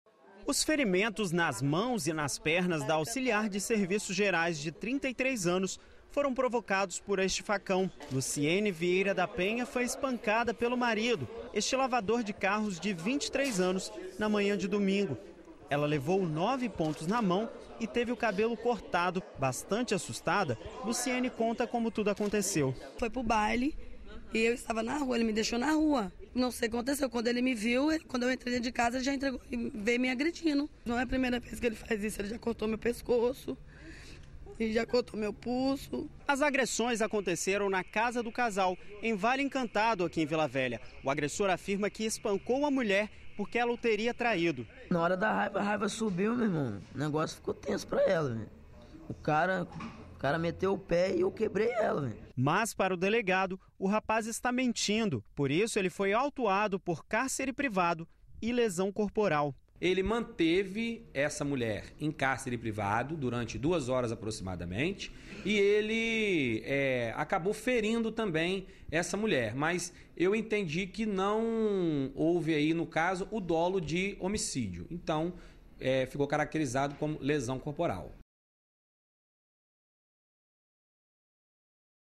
Mulher é espancada pelo marido
A mulher foi feita refém por horas, teve o cabelo cortado a facão, foi espancada e torturada com cortes no corpo. O agressor? O próprio marido. Um lavador de carros que justificou a brutalidade com frieza e acabou na cadeia.